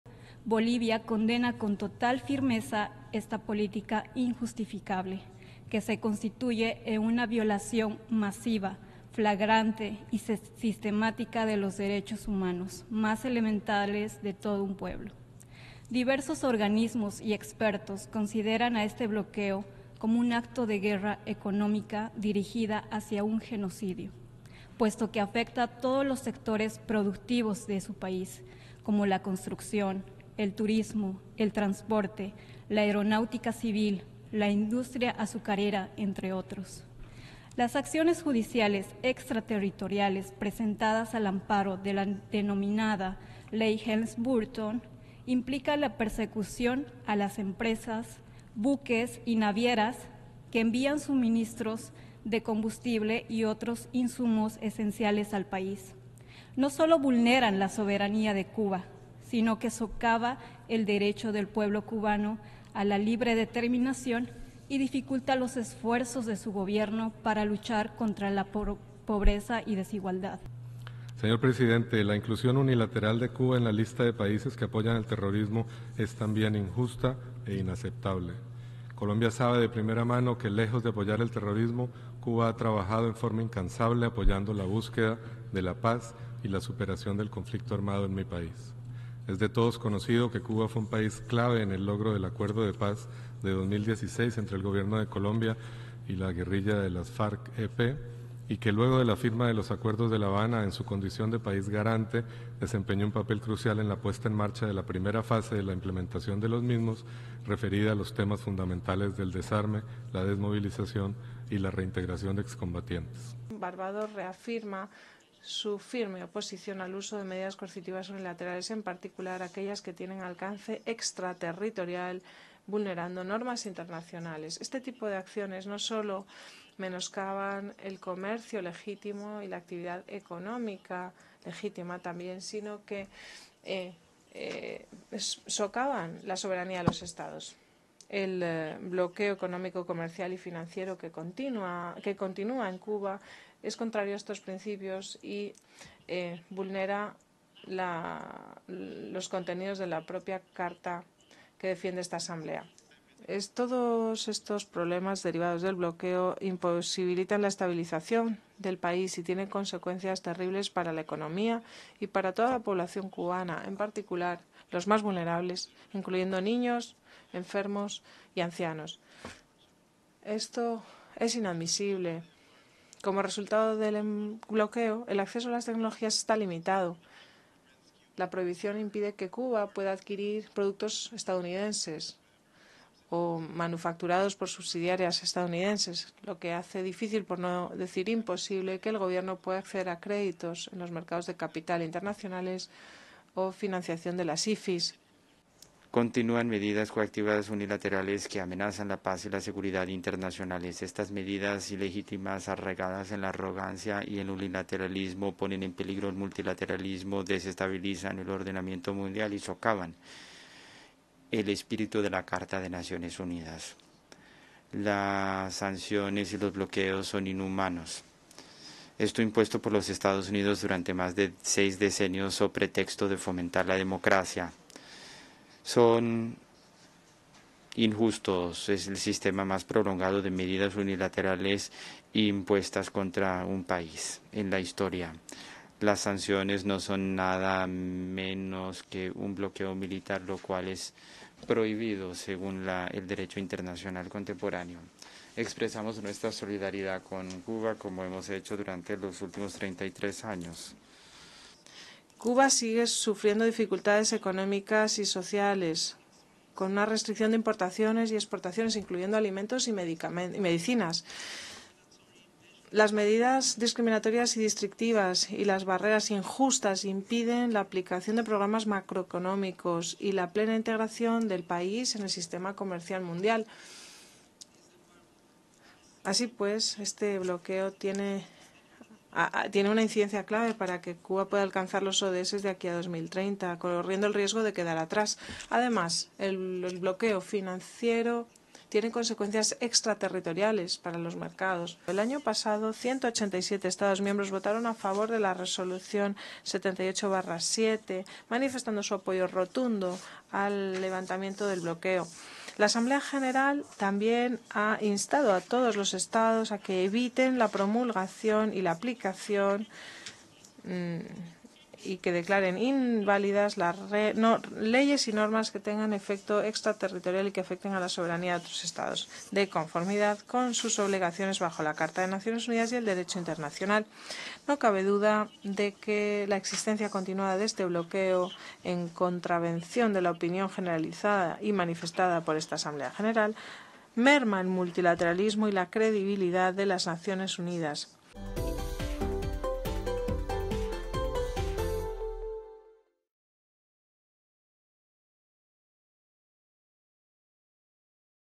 Debaten en la ONU proyecto de Resolución contra el Bloqueo
La sede de Naciones Unidas devino tribuna nuevamente contra el bloqueo de Estados Unidos a Cuba durante la primera jornada del 79 periodo de sesiones de la Asamblea General de la ONU. Decenas de representantes de los Estados miembros subieron al podio y rechazaron la aplicación de esa política de asfixia económica, comercial y financiera contra la mayor de las Antillas.